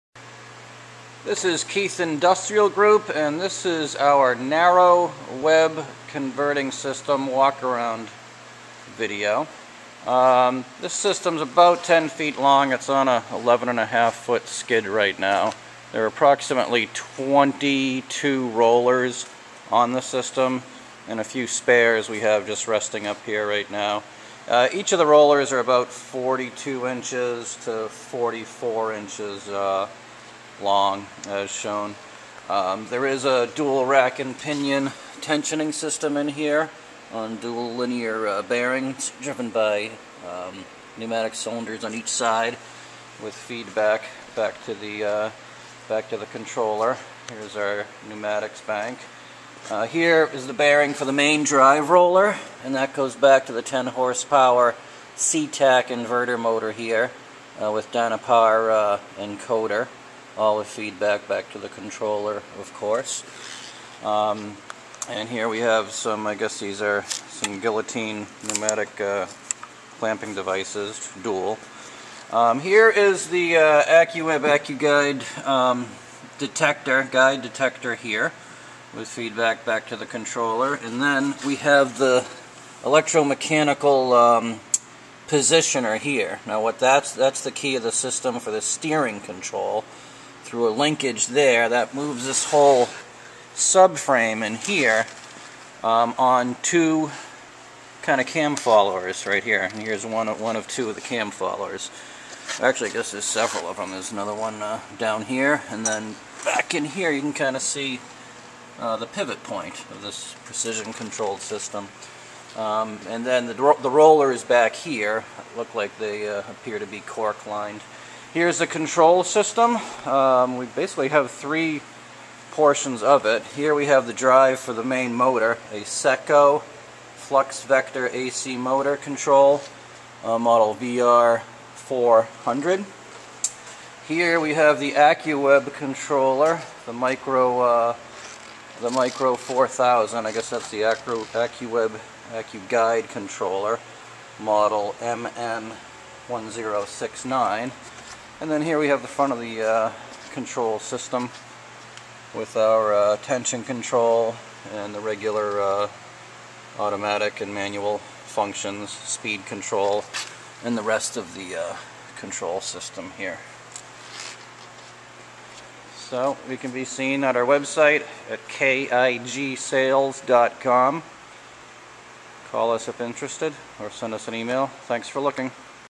For Sale: Accuweb Accuguide Narrow Web Converting Winder Unwinder Guide Detector Machine
Here we have a complete Narrow Web Converting Process Machine using some key primary components from Accuweb / Accuguide.
This machine was dismantled recently and removed from a laboratory R&D environment here in Massachusetts and then re-assembled here
at KIG, Inc's facility nearby in prepration for re-sale.  The machine consists of following key components:
*  heavy steel sideframe.  Precision machined with alignment bushings for re-assembly.
*  multiple very expensive rollers (some brand new).  Some spares are shown in photos resting on the top
*  10 hp Inverter Motor with Encoder and Inverter Drive
*  Dual rack and pinion web tensioning system controlled from control system
*  Accuweb / Accuguide Positive Displacement Guide System / Steering Roll System
*  Accuweb / Accuguide Edge Detector
*  Accuweb / Accuguide Electromechanical Precision Linear Actutor to Control Guide System
*  Accuweb / Accuguide Micro 4000 Web Controller System
.The system is on a custom skid, 5 ft wide x 11.5 ft long, and we can load onto truck free of charge. Further details are below, and link for walkaround video of the system on youtube. Helpful links are given below to Accuweb's current similar offering(s):

For this auction, the Steering frame model number and definition is below.  The very last photo we have shows one of two cam follower guide mechanisms with are an integral part of this "steering" technology when coupled with the actuator.
Model Nomenclature: 422402 PDG  s/n 1556 =
42 = 42 inch roll face
24 = 24 inch correction span
02 = 2 inch total correction

AccuGuide Postive Displacement Guide System

AccuWeb Electromechanical Linear Actuator.
Linear Actuator Model Number MN-3 7091-1   (this is the 4th Photo)
Motor for this above actuator is MTR 3094  Serial Number 3456
helpul link 3 (AccuWeb Current Acuators):

Accuweb Edge Detector    (This is the 3rd Photo)
Model 4x6U  4200  (U = Ultrasonic Type)

Micro 4000 Accuweb Accuguide Controller
Model from Cabinet is MN 1069  Serial number 3085
helpful link 5 (for current Micro 4000 Net Controller):
10 Hp CTac 460 volt Inverter AC Motor which used Dynapar Model HA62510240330 Encoder
The Motor is controlled by the Secofluc Vector Drive within the cabinet, Model VR400.